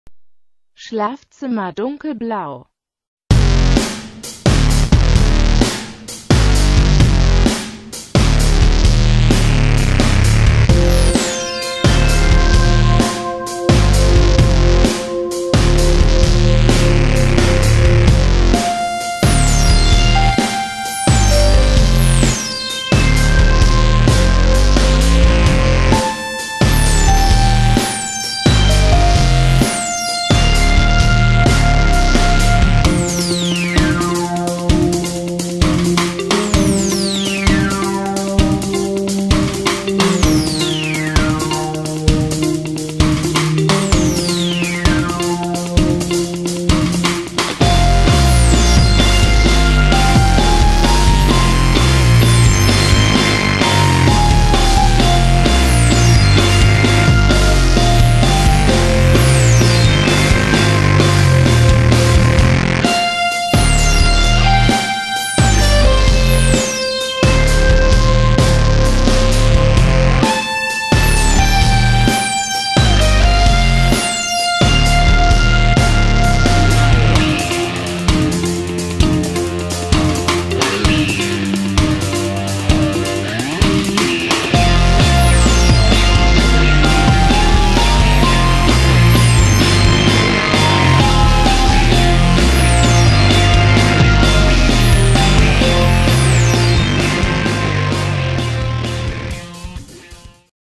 Schlafzimmer dunkelblau
Das Video zeigt schlafzimmer dunkelblau.

Dunkelblau gilt als die beste Farbe für das Schlafzimmer zu sein. Es wird empfohlen, und Designer und Psychologen und sogar Ärzte. Es wird behauptet, dass im Schlafzimmer blauen Farbeffekte hat Heilung: Er verjüngt, senkt den Blutdruck, entspannt sich, als leichte Massage und den Traum von höherer Qualität zu machen. Viele unserer Mitbürger leben in einem kalten Klima, und denken oft nicht einmal über eine dunkelblaue Töne im Inneren seines Hauses verwenden, diese Farbe zu glauben, zu kalt und dunkel. Aber es war im Schlafzimmer, diese Eigenschaften mehr als angemessen.